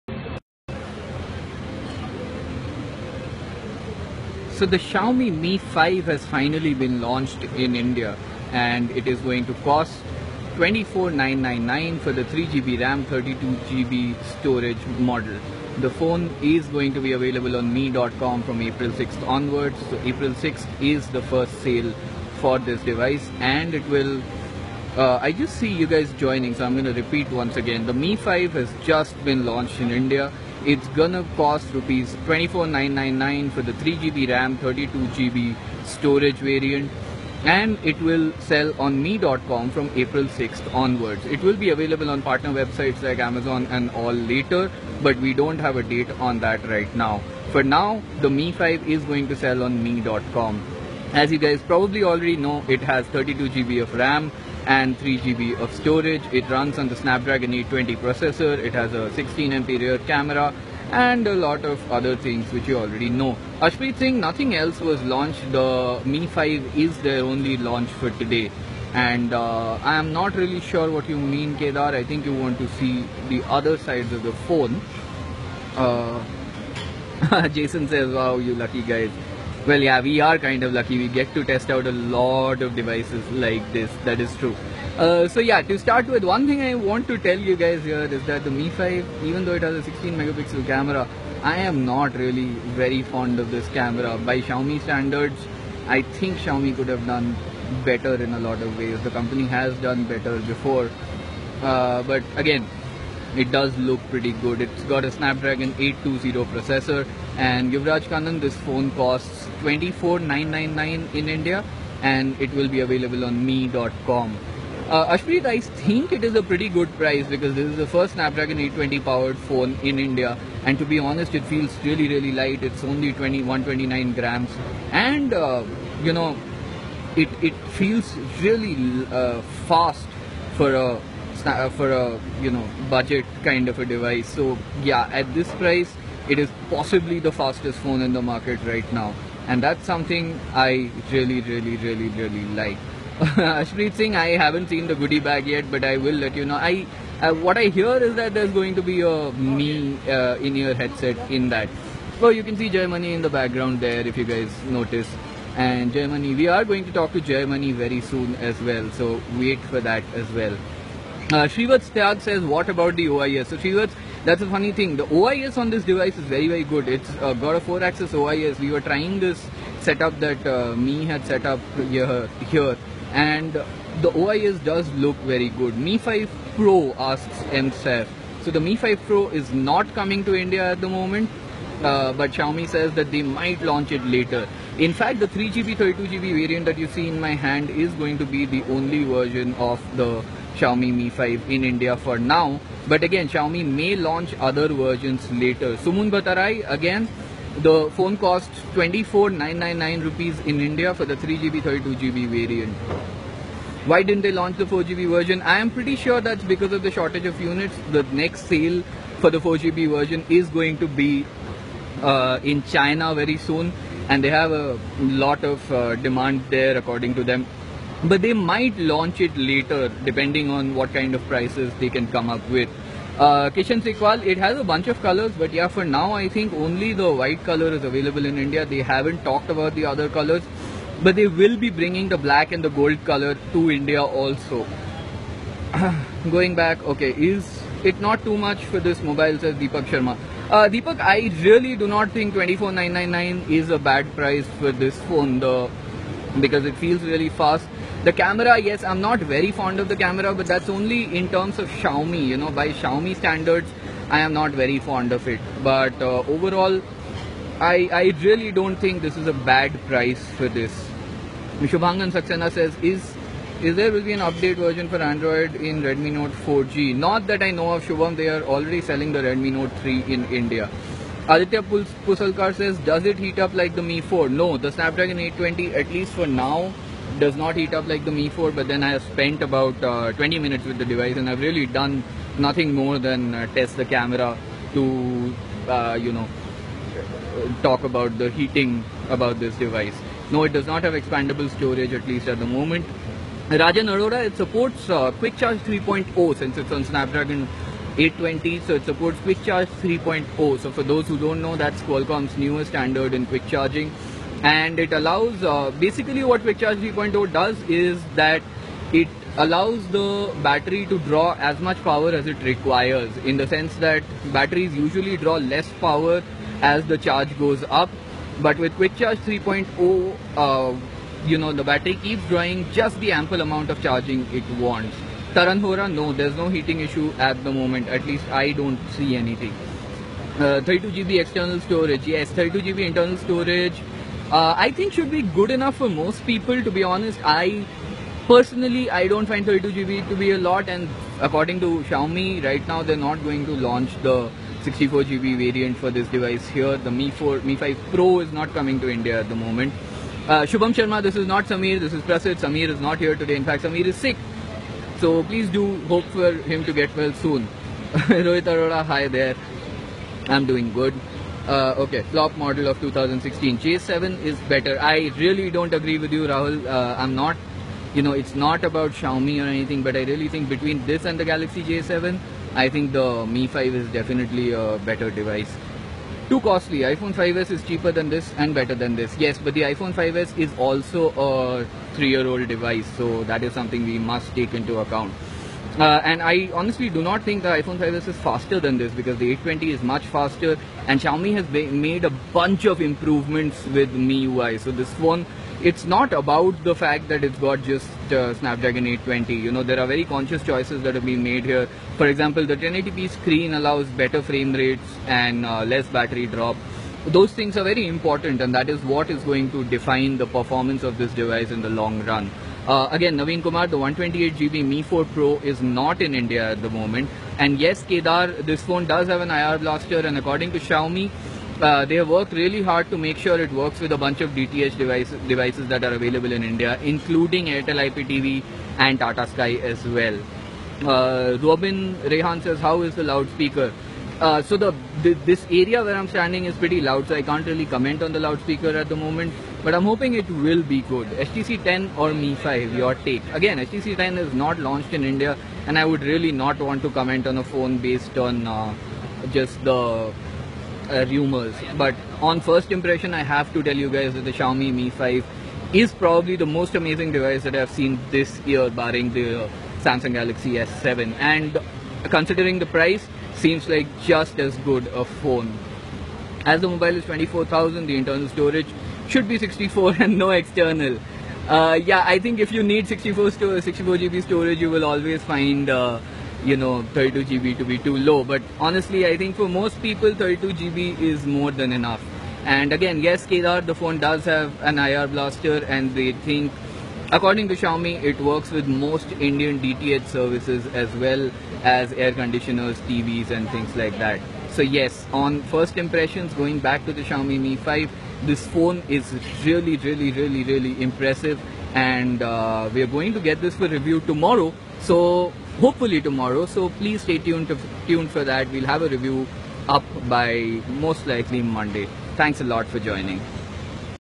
Xiaomi Mi5 | Hands On Review | Mi 5 question answer
Xiaomi Mi 5 at a glance: Snapdragon 820 @ 1.8GHz, 3GB RAM, 32GB internal storage, 3000mAh battery pack, 16MP primary camera with 4-axis OIS and DTI, IR Blaster, available on mi.com from April 6, price Rs. 24,999.